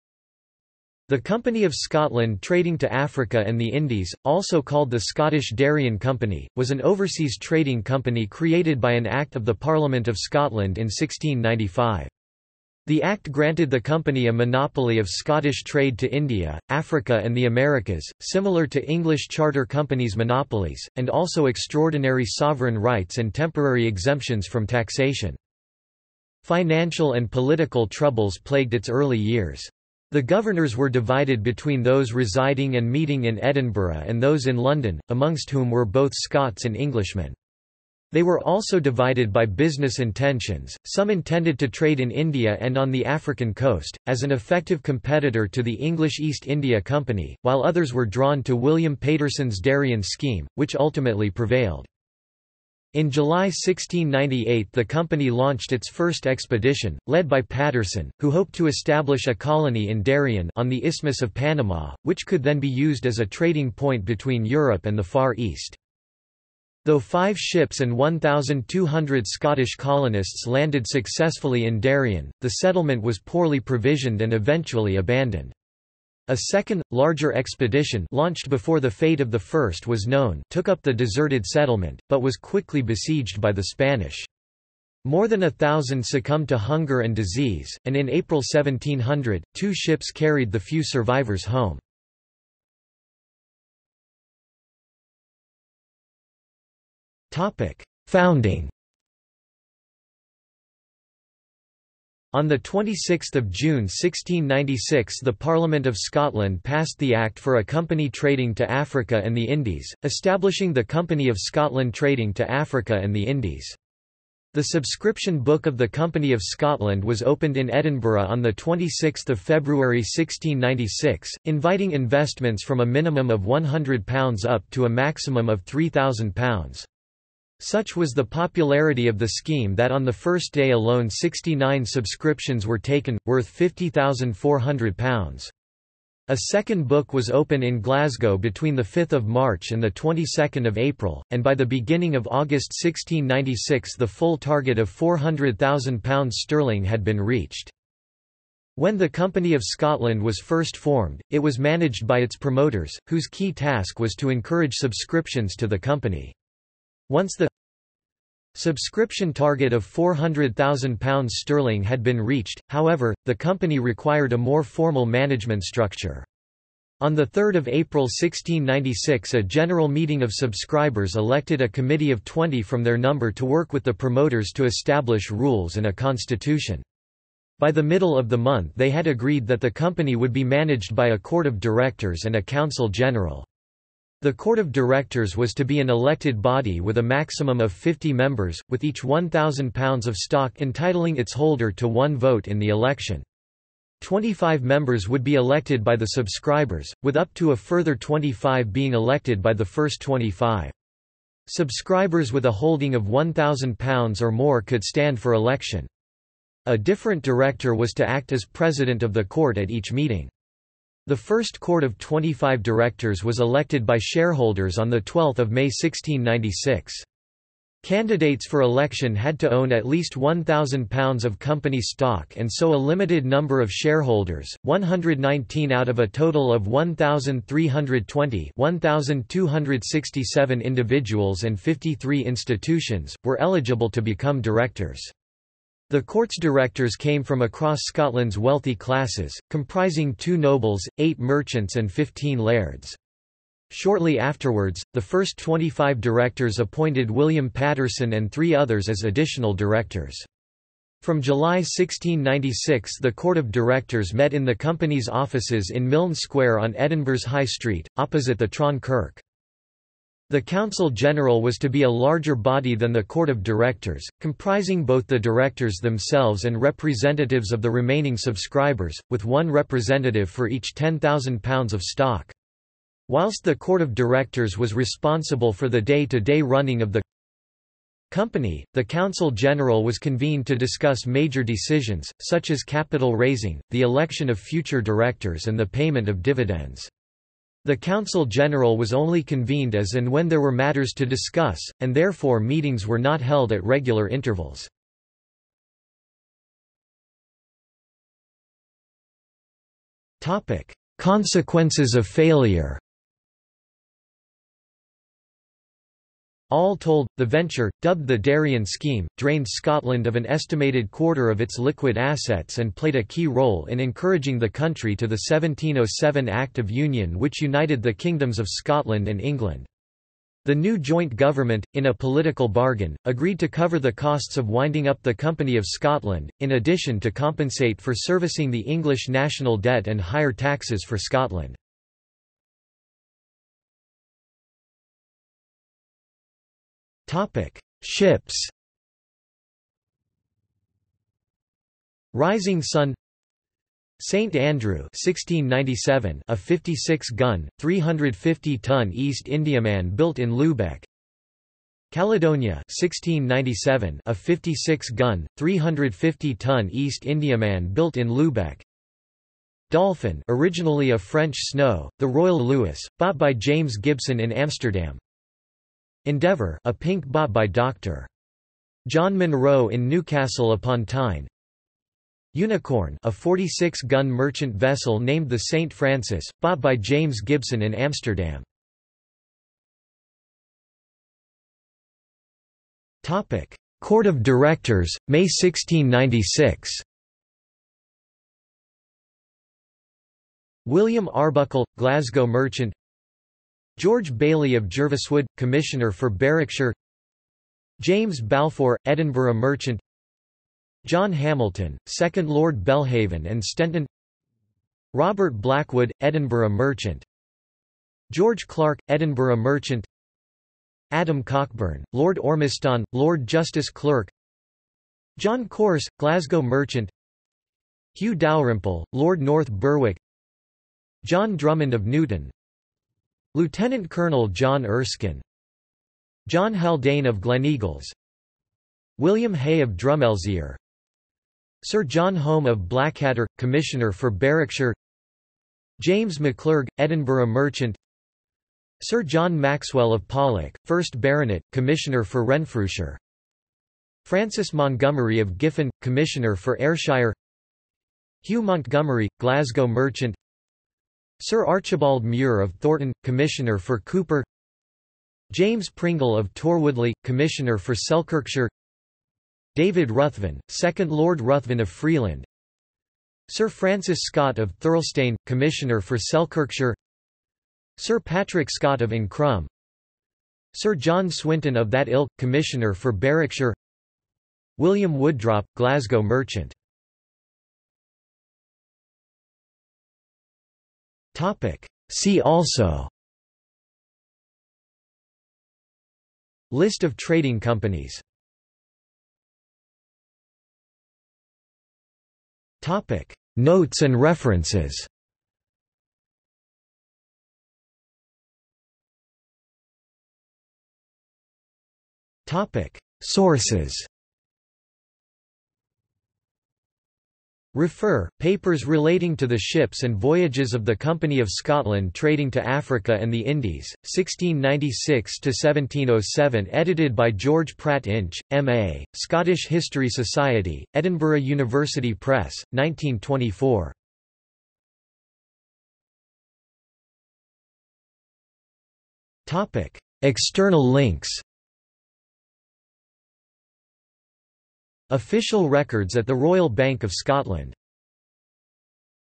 Company of Scotland | Wikipedia audio article
00:01:33 1 Founding
00:04:56 2 Consequences of failure
00:05:33 3 Ships
00:06:19 4 Court of Directors, May 1696
00:07:57 5 See also
00:08:09 6 Notes and references
00:08:18 7 Sources
00:08:45 8 External links

- Socrates

SUMMARY
The Company of Scotland Trading to Africa and the Indies, also called the Scottish Darien Company, was an overseas trading company created by an act of the Parliament of Scotland in 1695. The Act granted the Company a monopoly of Scottish trade to India, Africa and the Americas, similar to English charter companies' monopolies, and also extraordinary sovereign rights and temporary exemptions from taxation.
Financial and political troubles plagued its early years.  The governors were divided between those residing and meeting in Edinburgh and those in London, amongst whom were both Scots and Englishmen.  They were also divided by business intentions; some intended to trade in India and on the African coast, as an effective competitor to the English East India Company, while others were drawn to William Paterson's Darien scheme, which ultimately prevailed.
In July 1698 the company launched its first expedition, led by Paterson, who hoped to establish a colony in Darien (on the Isthmus of Panama), which could then be used as a trading point between Europe and the Far East.
Though five ships and 1,200 Scottish colonists landed successfully in Darien, the settlement was poorly provisioned and eventually abandoned. A second, larger expedition (launched before the fate of the first was known) took up the deserted settlement, but was quickly besieged by the Spanish. More than a thousand succumbed to hunger and disease, and in April 1700, two ships carried the few survivors home.